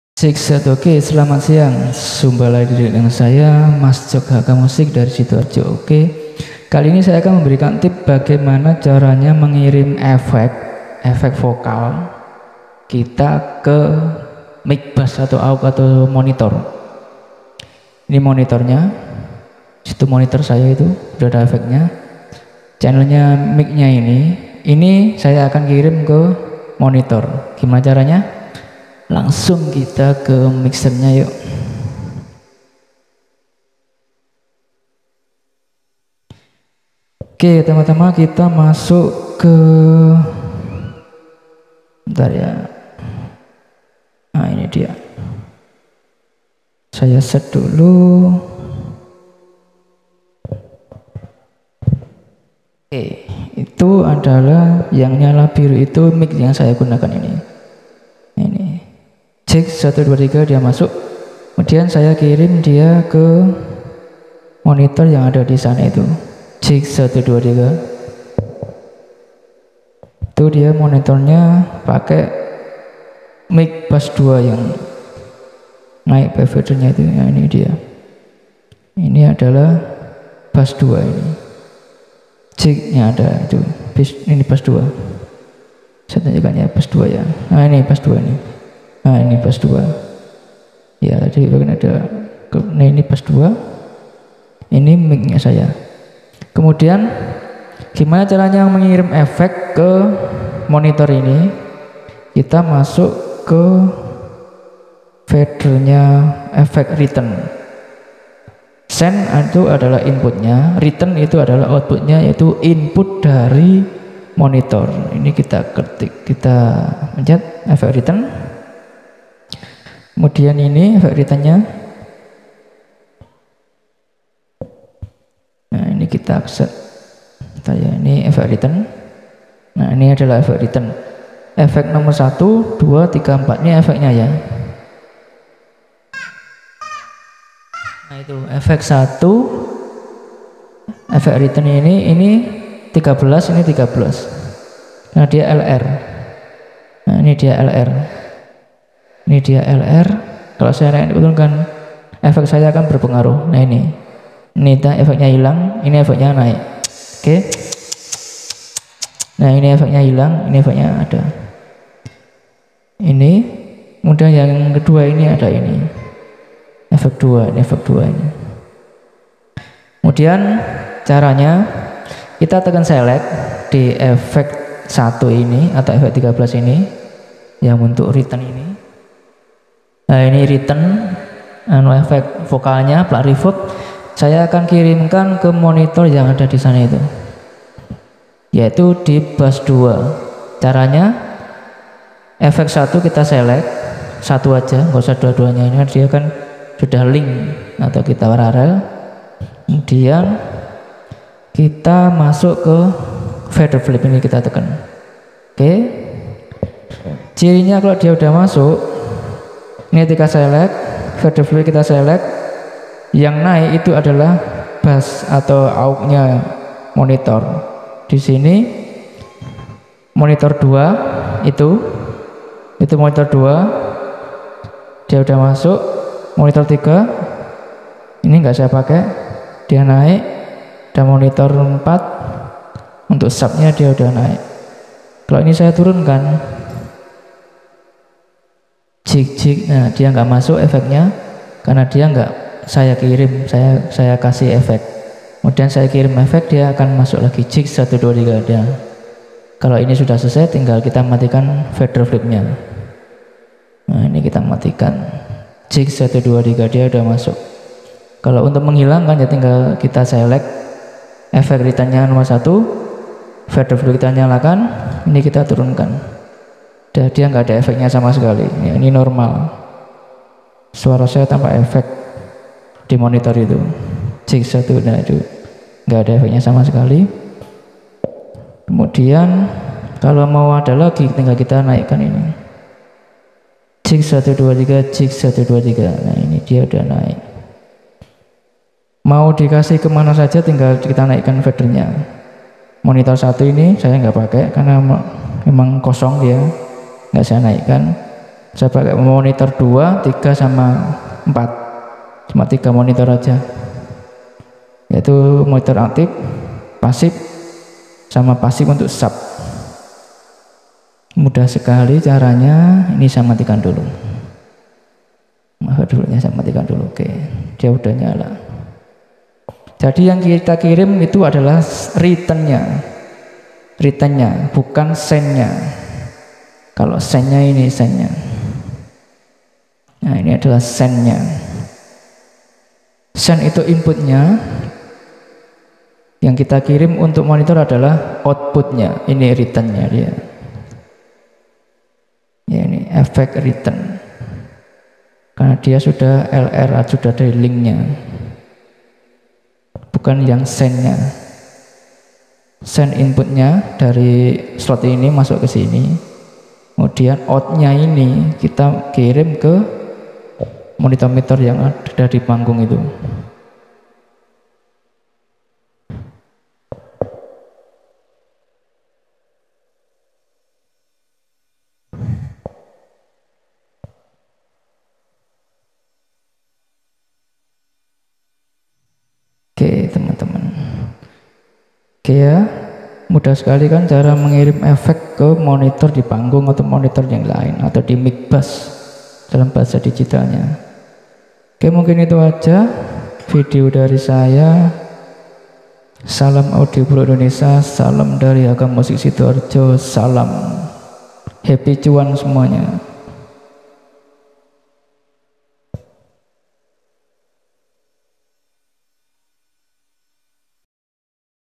Kirim efek ke mix bus aux atau monitor di mixer MIDAS M32
Cara kirim effect ke mix bus aux atau monitor di mixer digital midas M32